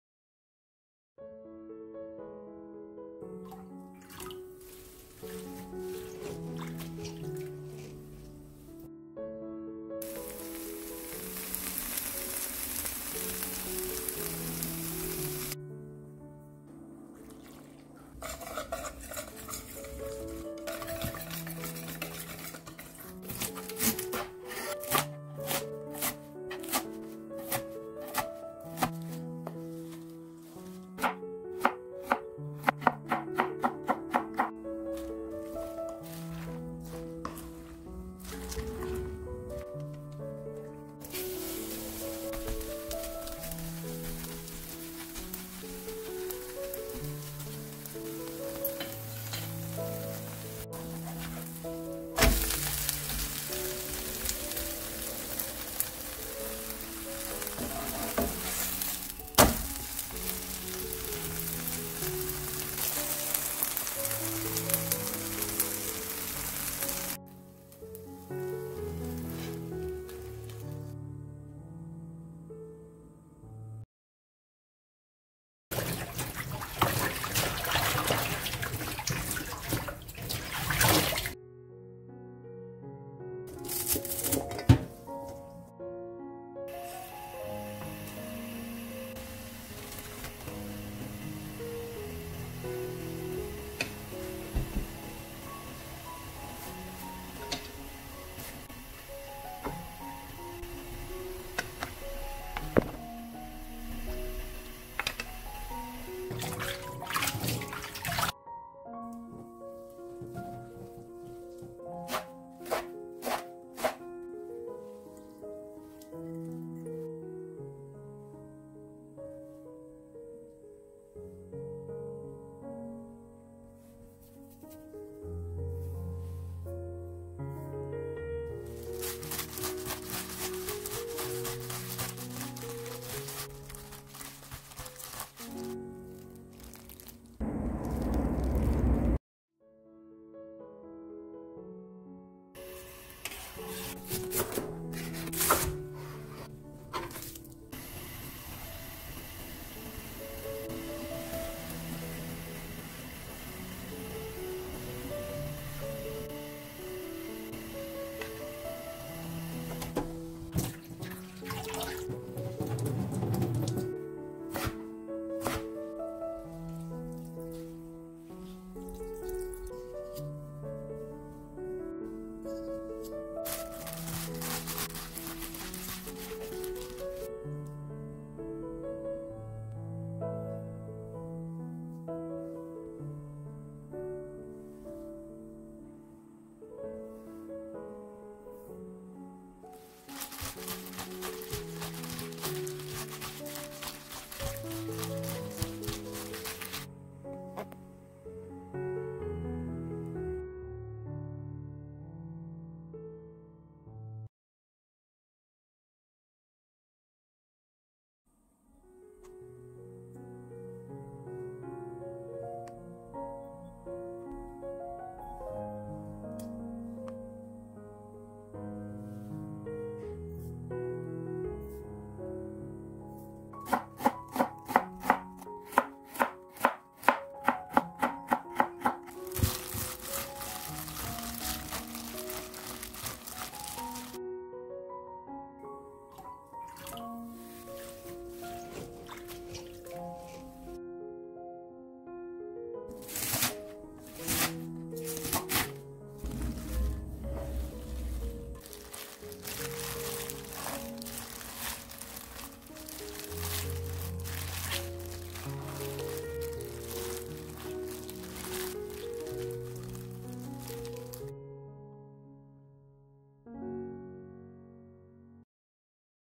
미나리 4가지요리 미나리전 미나리나물 미나리겉절이 미나리초무침
미나리 4가지요리

미나리전 00:15
미나리나물 01:13
미나리초무침 02:21
미나리겉절이 03:28